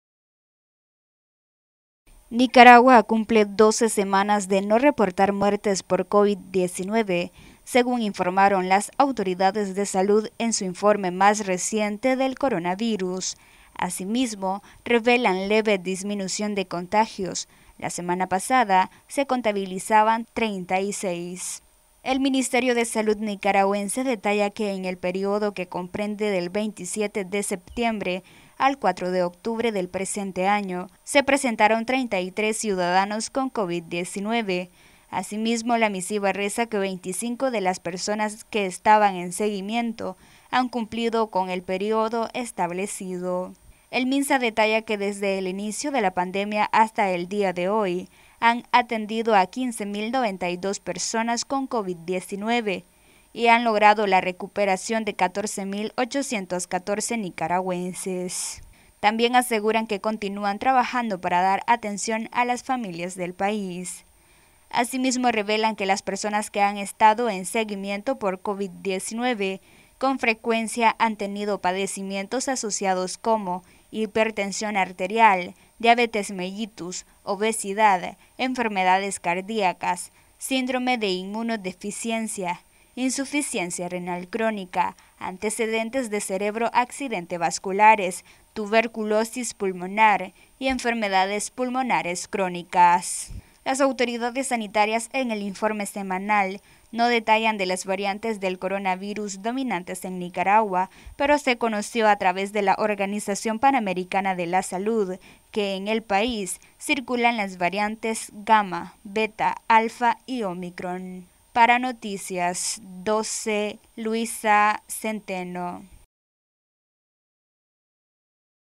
Noticias12NicaraguaMinsa reporta disminución de contagios por Covid-19 en la última semana
Nicaragua reporta disminución de contagios de Covid-19 en la última semana